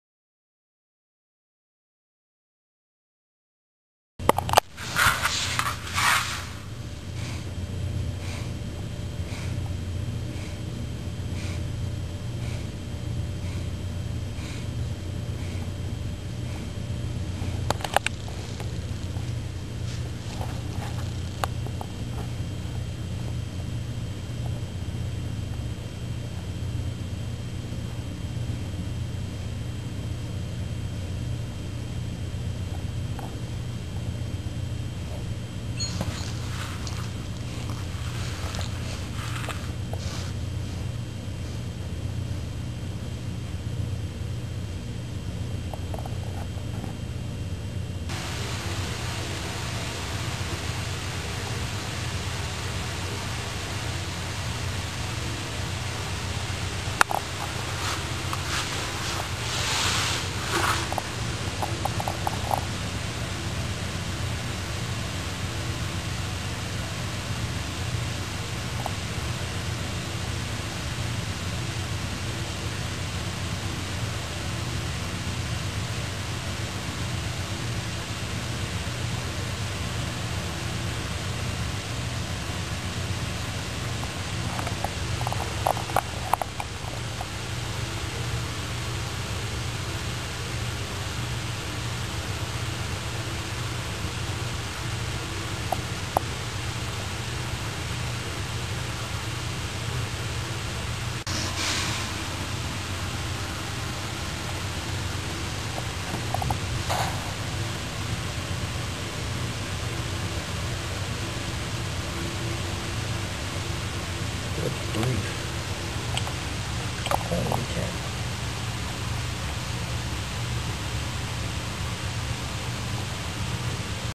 Hemi's active baby bump!
Hemi is due any day now and the babies have been very active!  We caught some of the movement on video to share.  You do have to watch carefully, but once you spot the puppy kicks, they can be hard to miss.

I guess you can see she is panting a lot even though the room is only 70 degrees, this is a real indicator of how much extra effort it is hauling around 7 baby puppies.

It may look as though we've shaved her belly, but the hair has been falling out naturally as she gets ready to nurse.

This is Hemi's first litter and ours.  We are excited, but she's still content to sleep and keep us waiting on the edge of our seats!